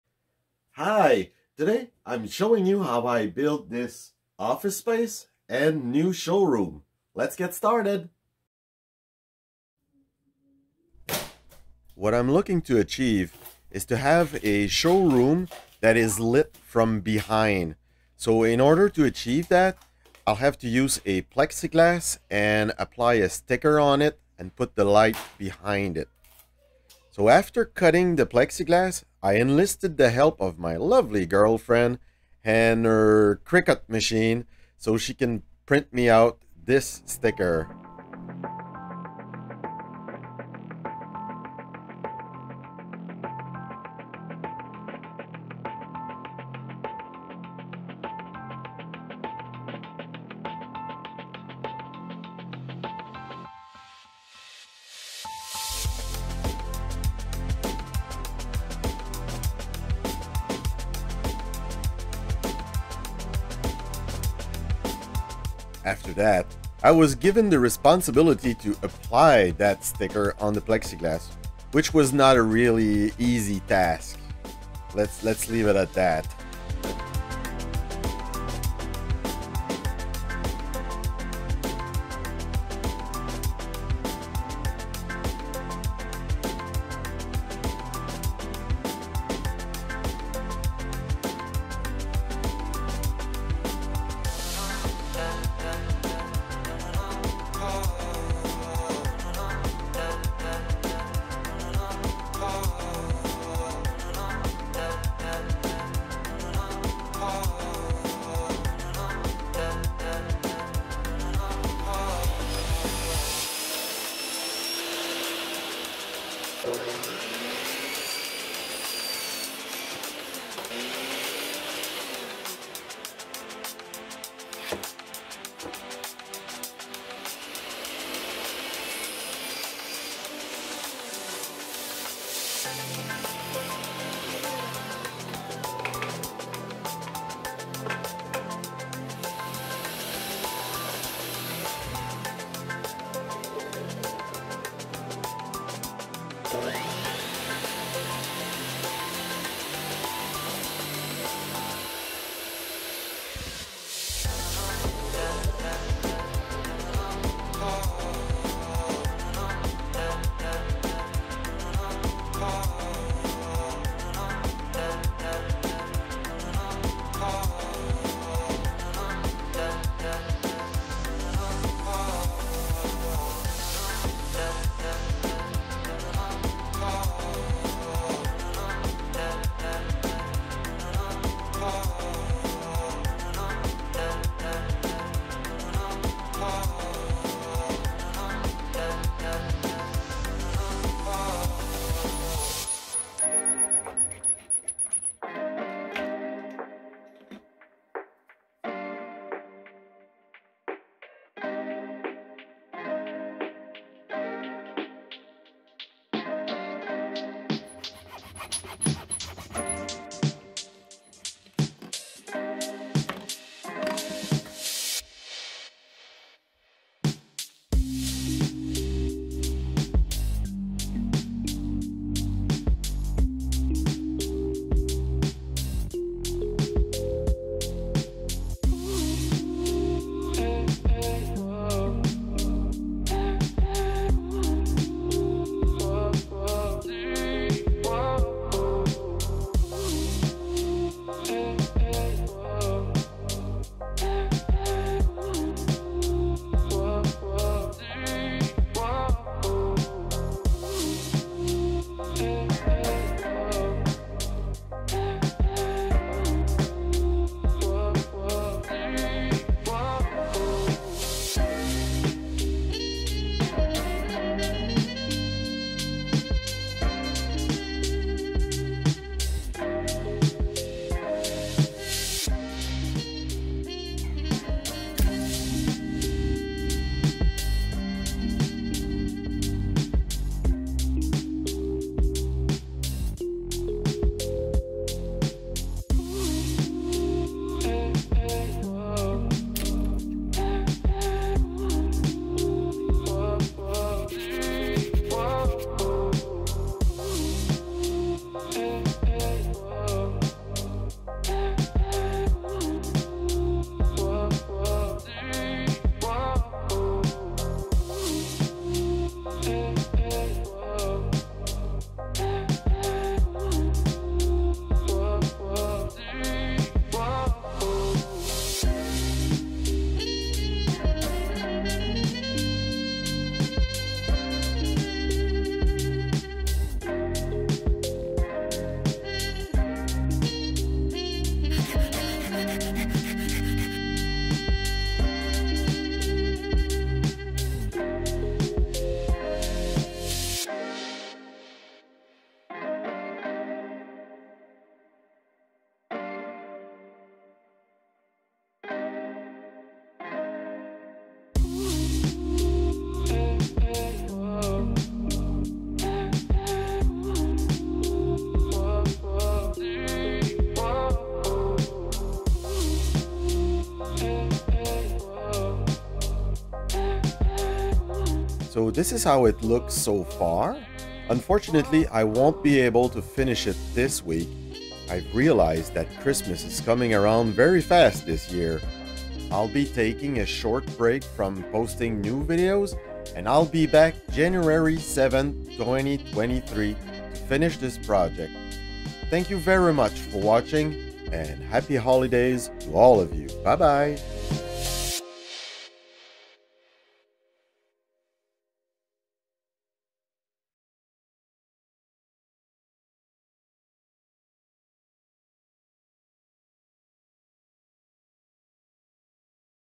Building 1/18 Scale Office Space and Showroom
Hi, in this new video, I will show you how I build a 1/18 scale office for the boss and a new showroom for my 60"x24" showroom diorama.